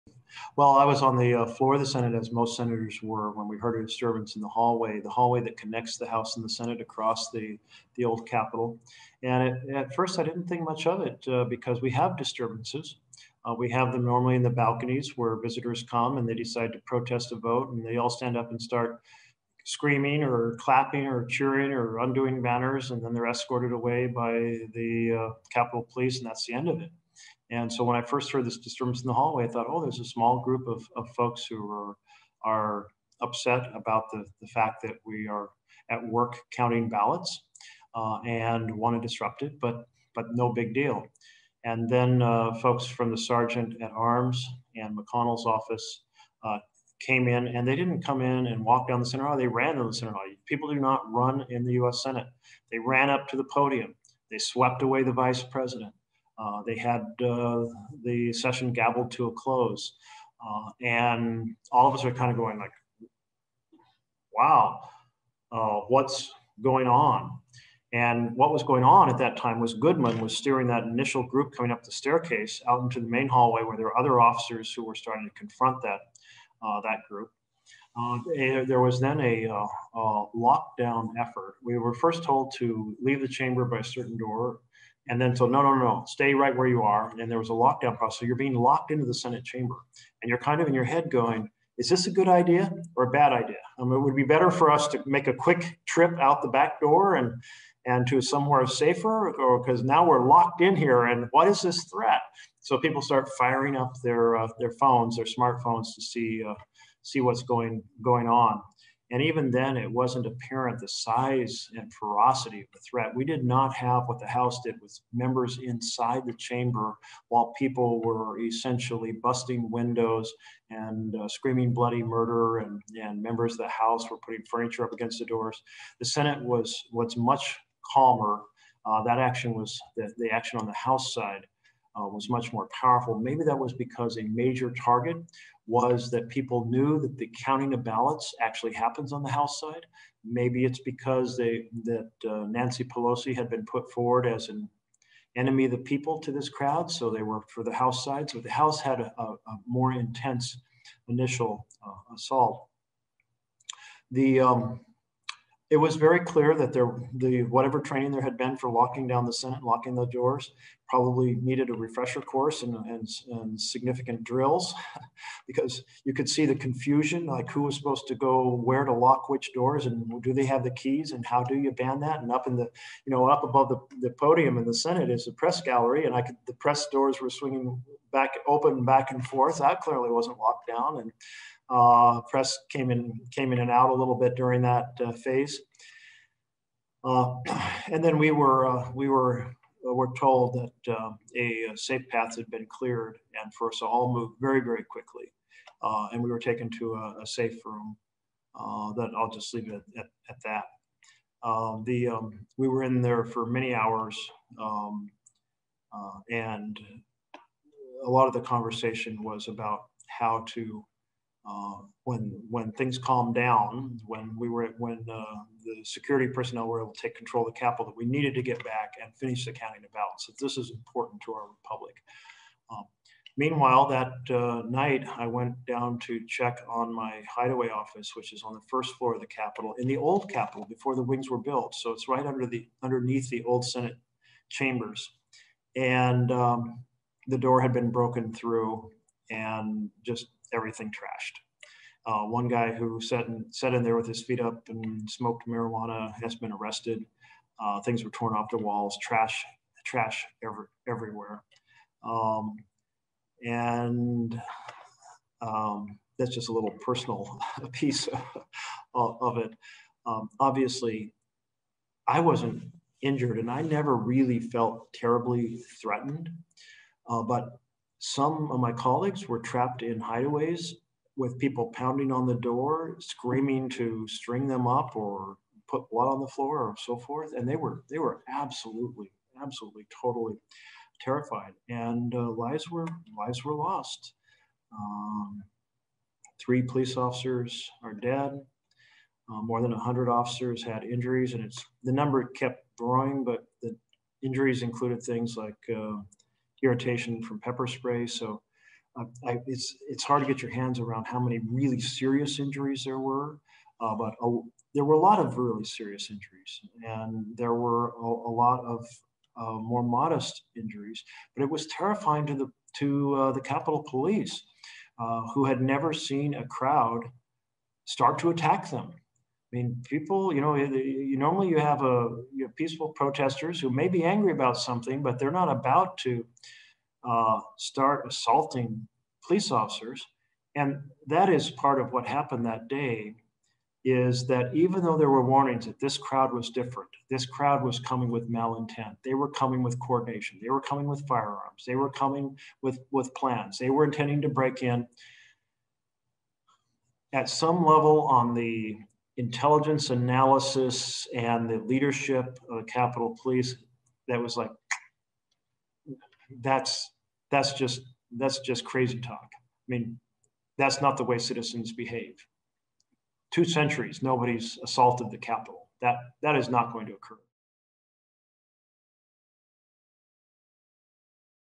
Sen. Jeff Merkley details his experience during the January 6th Capitol riot
During an interview with The Oregonian/OregonLive Editorial Board, U.S. Sen. Jeff Merkley of Oregon describes what it was like to be at the U.S. Capitol in Washington, D.C. during the January 6th riots. Merkley was on the Senate floor when a mob sieged the Capitol in an attempt to overturn the results of the presidential election when Donald Trump lost to Joe Biden.